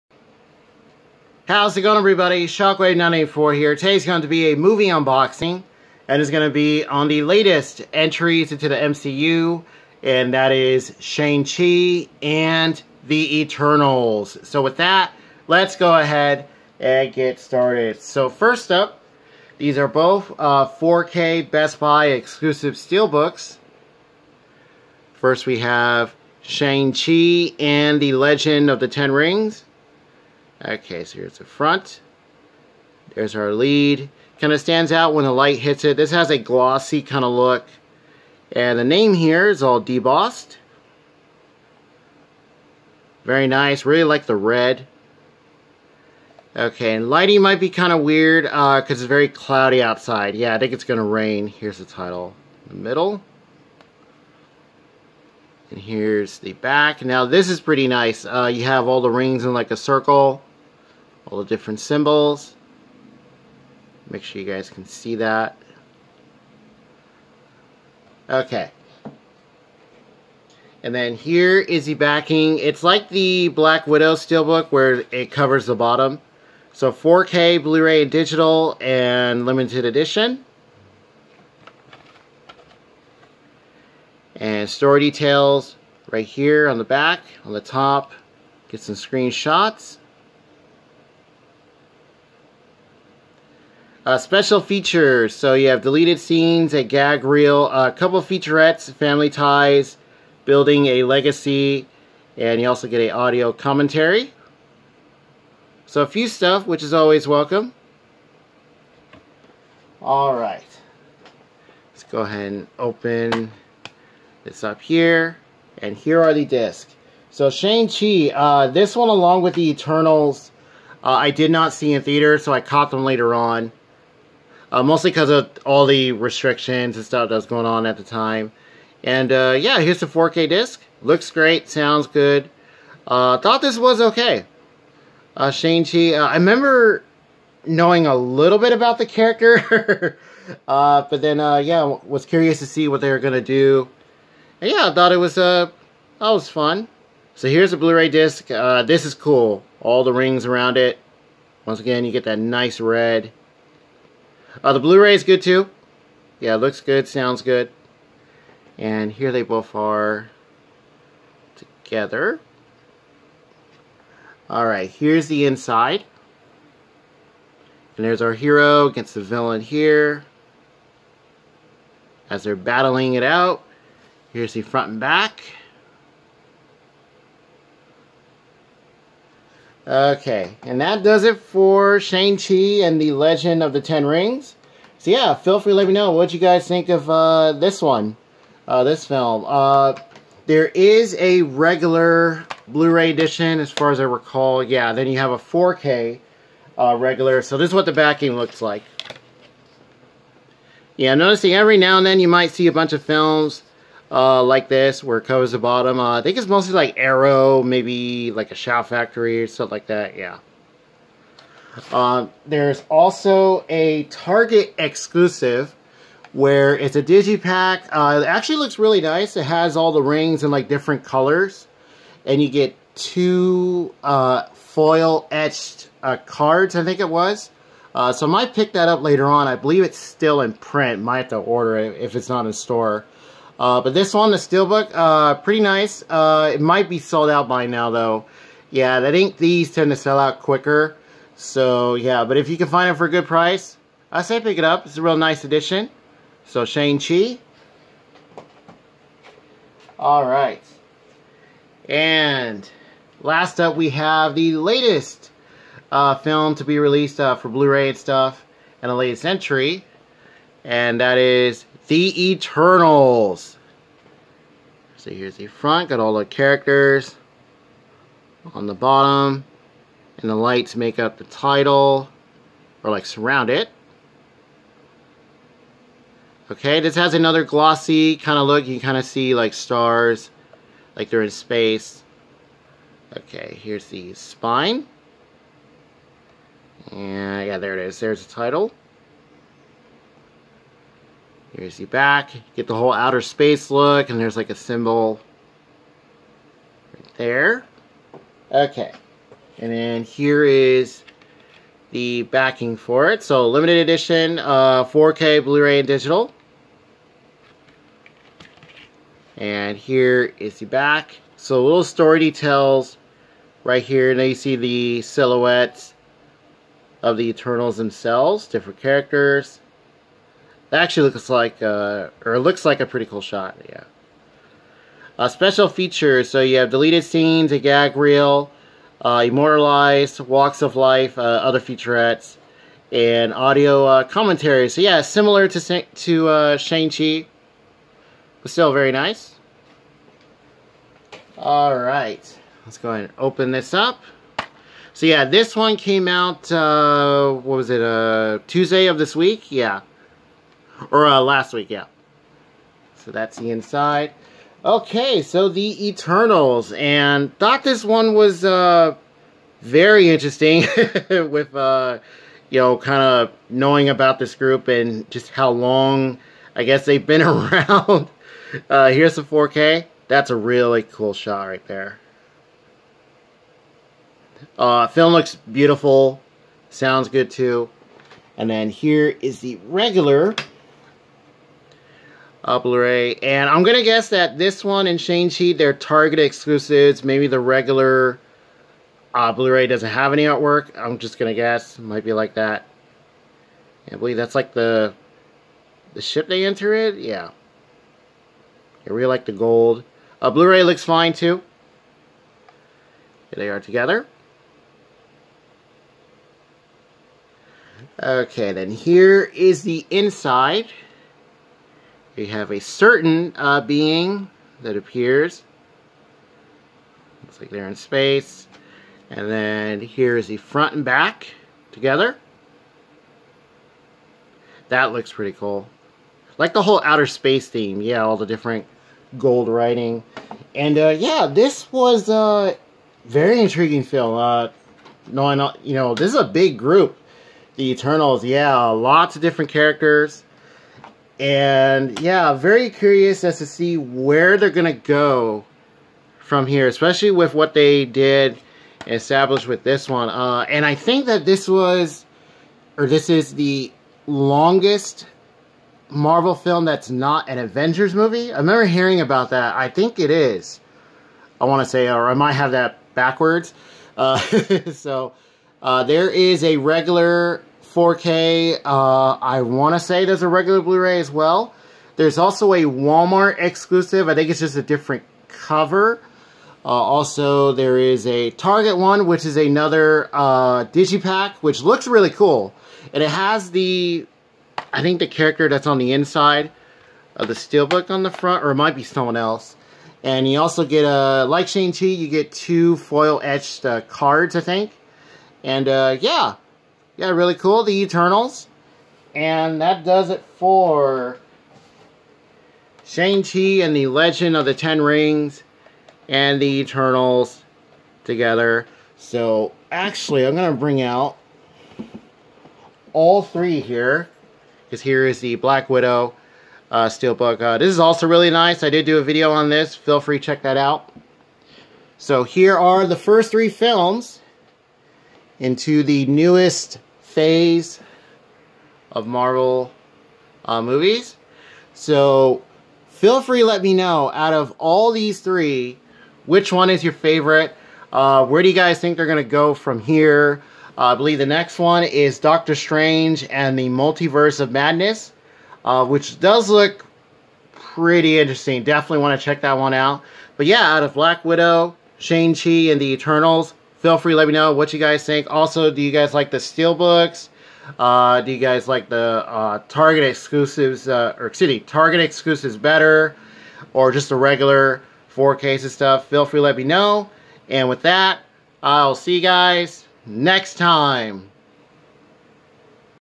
Unboxing Shang-Chi and Eternals Steelbooks
Just 2 items to show today for Shang-Chi and the Legend of the Ten Rings and the Eternals 4K steelbooks. These movies are really amazing in their own way and I would highly recommend checking them out.